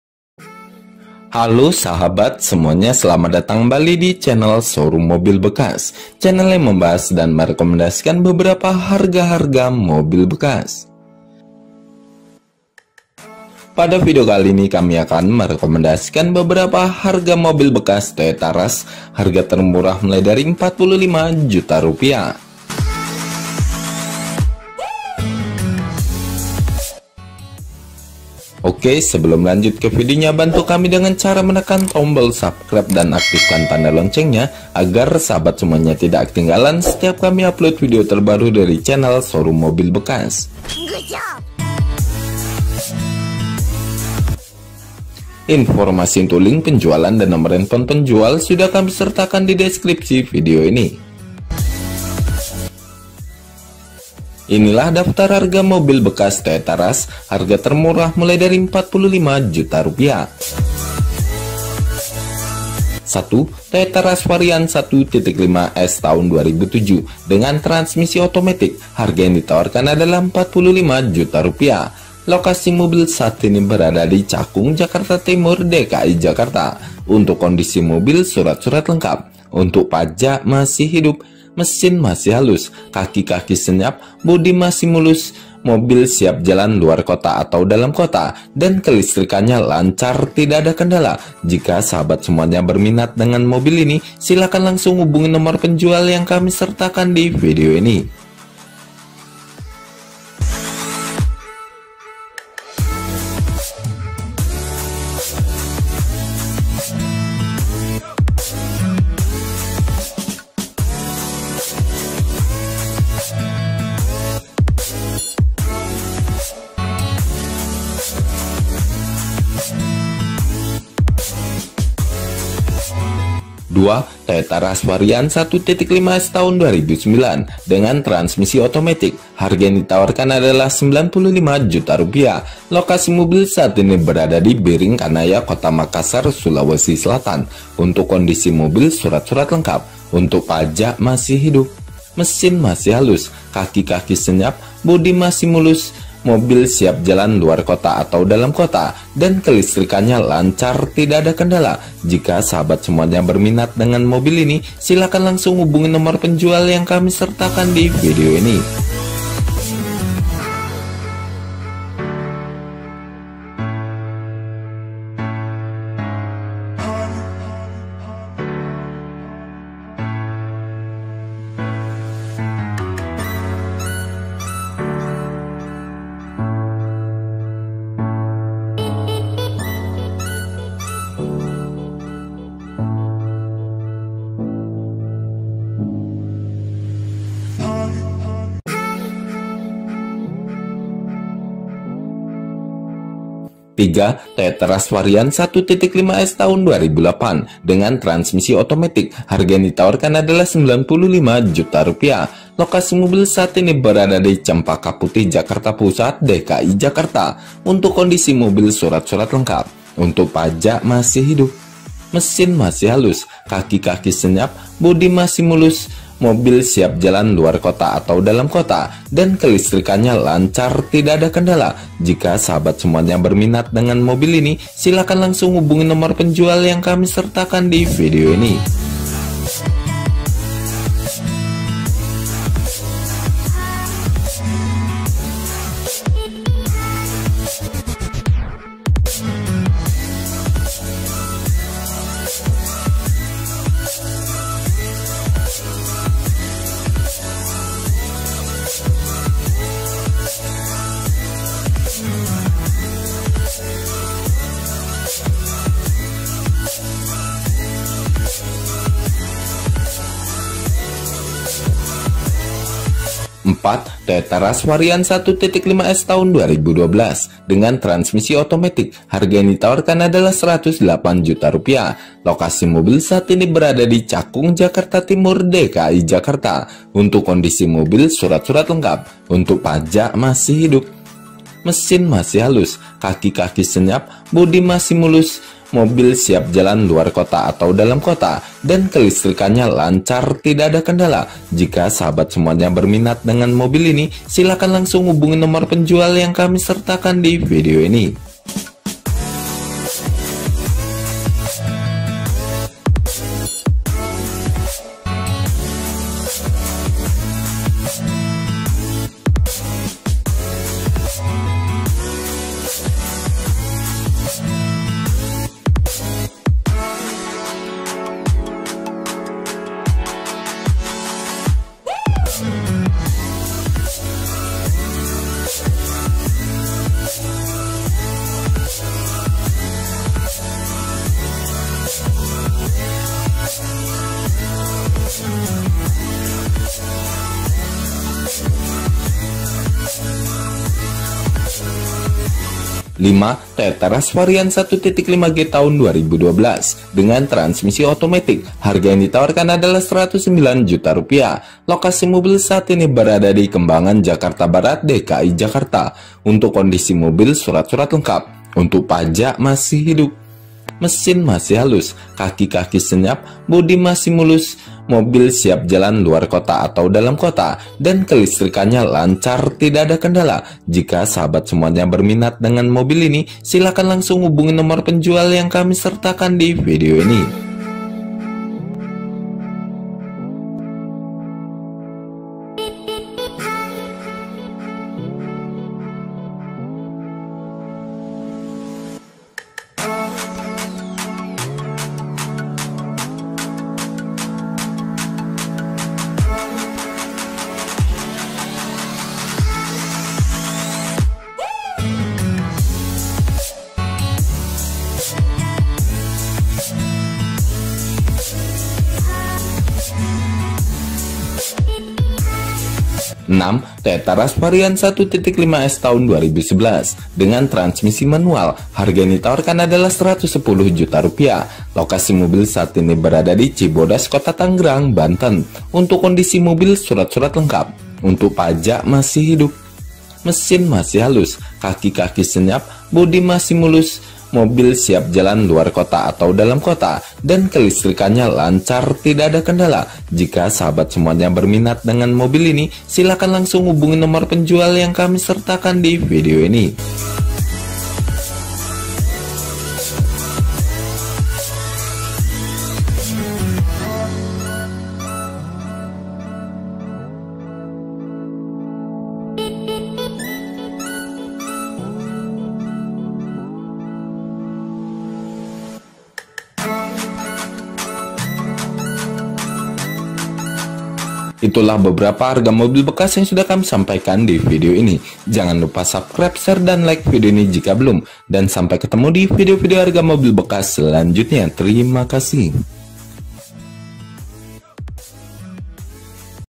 HARGA MOBIL BEKAS TOYOTA RUSH TAHUN 2007 - 2012 MURAH
HARGA MOBIL BEKAS TOYOTA RUSH

1. TOYOTA RUSH 1.5 S TAHUN 2007, HARGA 45 JUTA

2. TOYOTA RUSH 1.5 S TAHUN 2009, HARGA 95 JUTA

3. TOYOTA RUSH 1.5 S TAHUN 2008, HARGA 95 JUTA

4. TOYOTA RUSH 1.5 S TAHUN 2012, HARGA 108 JUTA

5. TOYOTA RUSH 1.5 G TAHUN 2012, HARGA 109 JUTA

6. TOYOTA RUSH 1.5 S TAHUN 2011, HARGA 110 JUTA

TERIMA KASIH